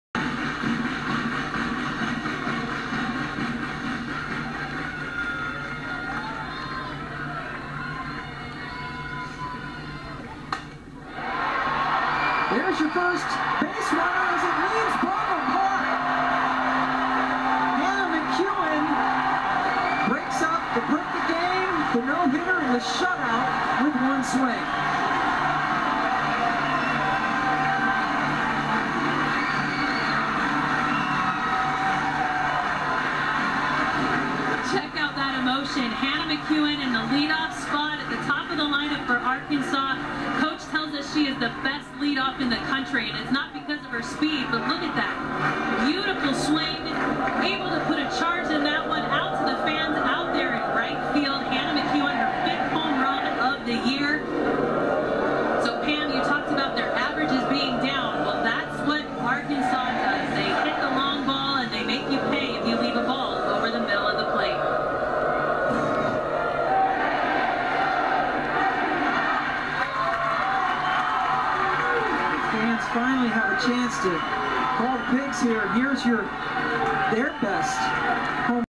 Arkansas softball's Hannah McEwen hits home run vs. Arizona in super regional, 5/28/21
Arkansas softball's Hannah McEwen hits a home run vs. Arizona in Game 1 of a best of 3 super regional, 5/28/21.